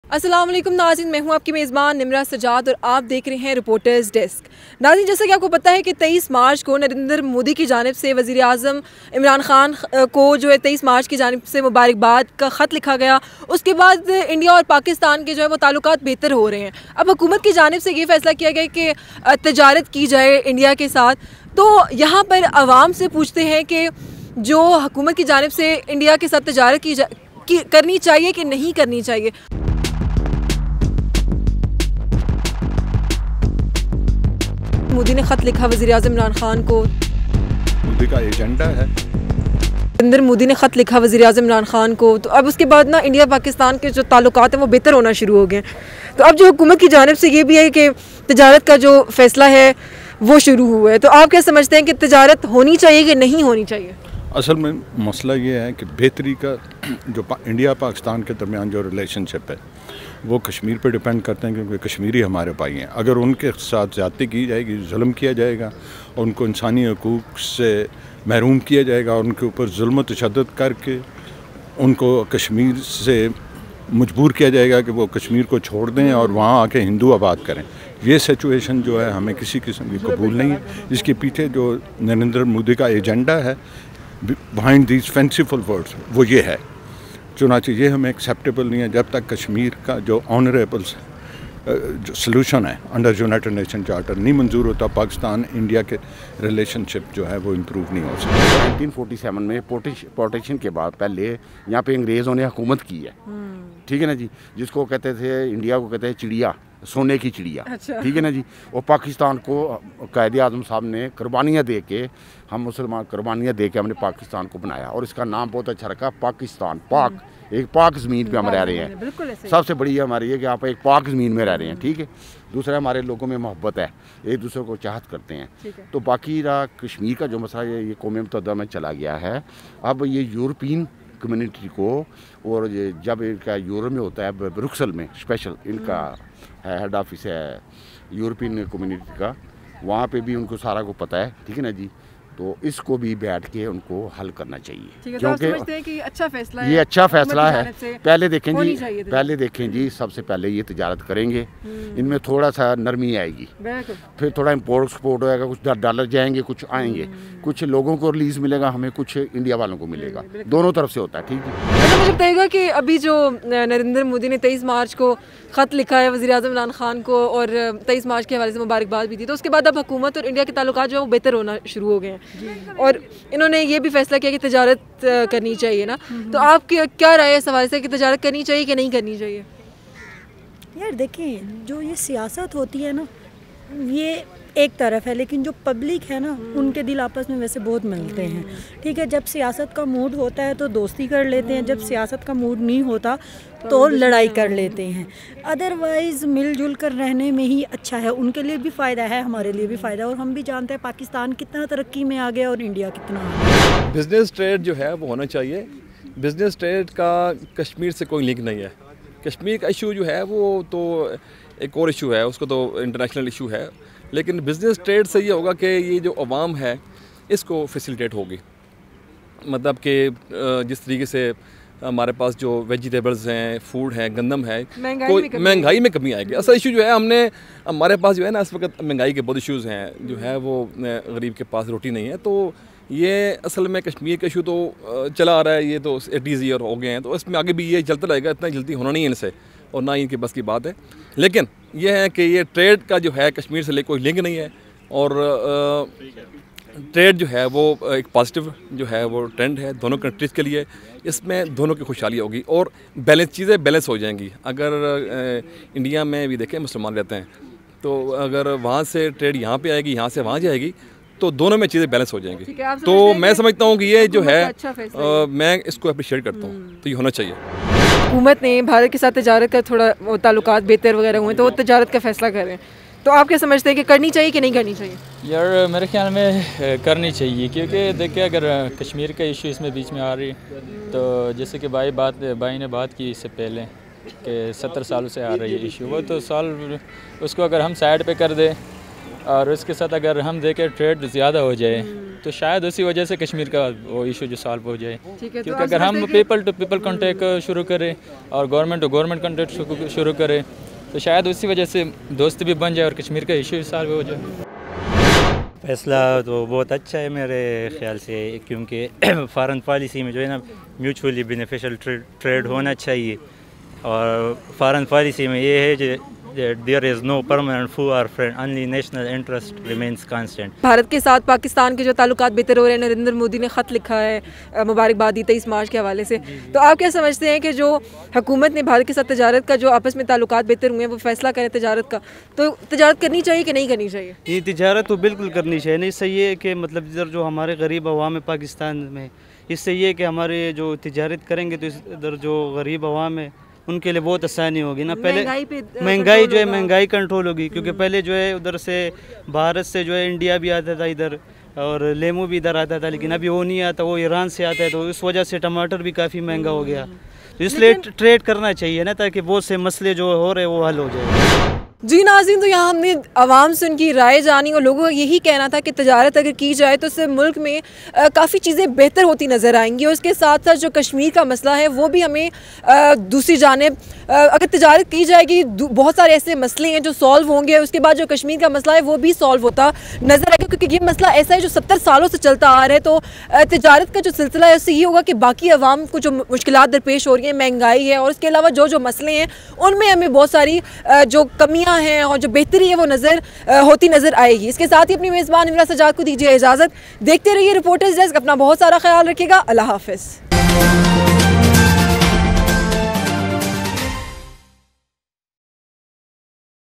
کیا مودی کوئی نئی چال چل رہا ہے؟عوام کیا کہتے ہیں؟ | Reporters Desk
مودی کا عمران خان کو خط
کیا مودی کوئی نئی چال چل رہا ہے؟
عوام کیا کہتے ہیں؟
People opinion about letter diplomacy between Modi and Imran Khan?